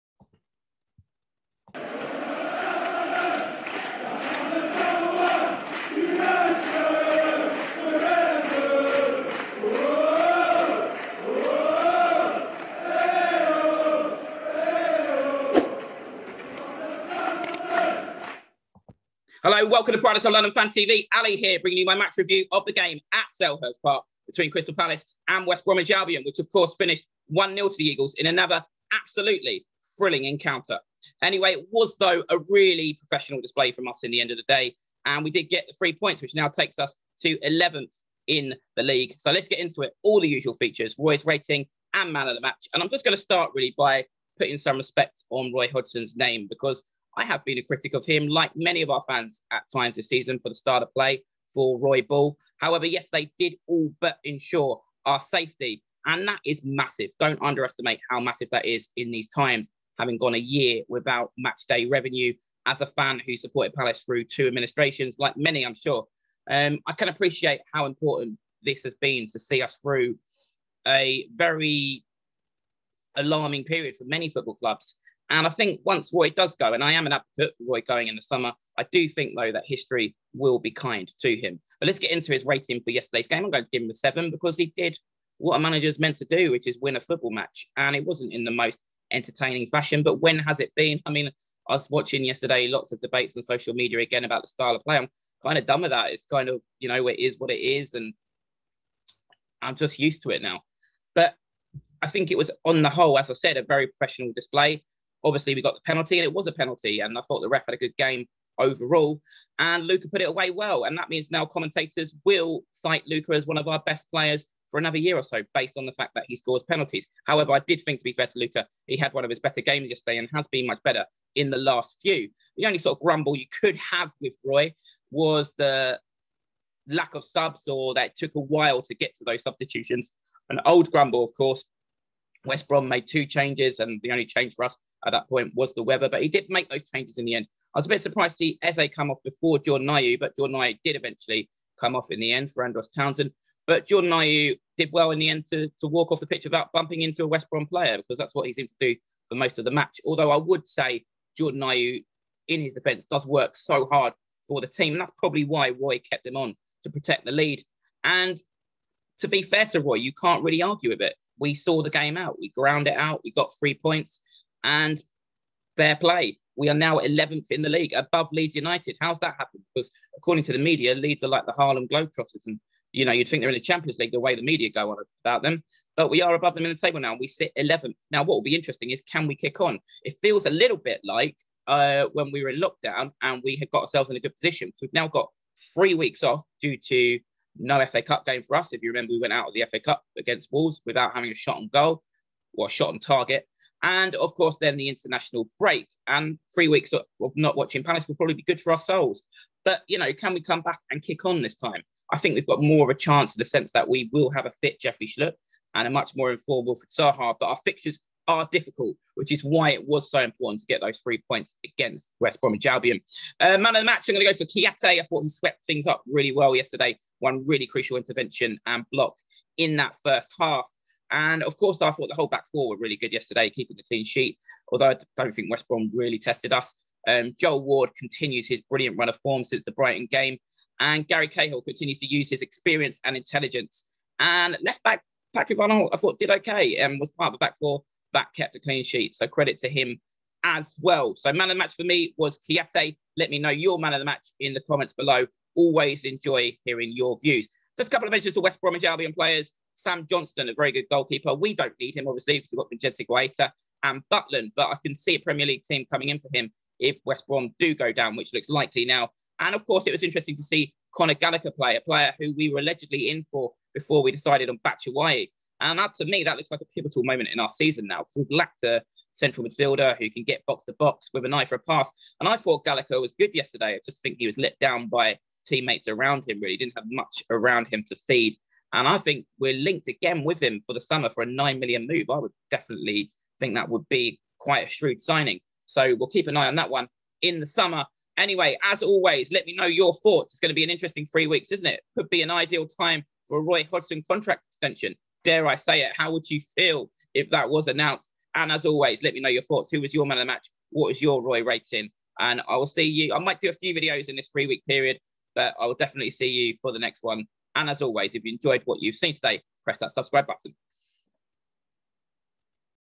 Palace 1 - WBA 0 review.
I review our 1-0 win over WBA, all but condemning them to relegation and ensuring our safety. Explain why I feel it is time to put some respect on Roy Hodgson's name. All the usual features:
-Roy's rating